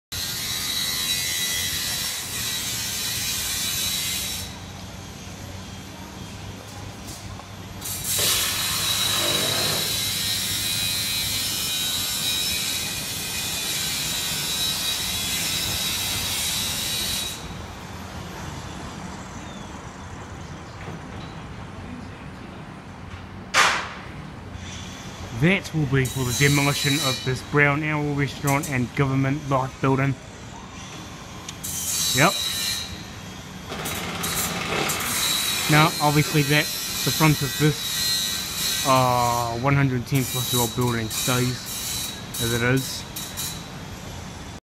Invercargill Block Demolition/Redevelopment Update Part 24 (For Fun Only)
These two big old abandoned looking buildings are now a step closer to demolition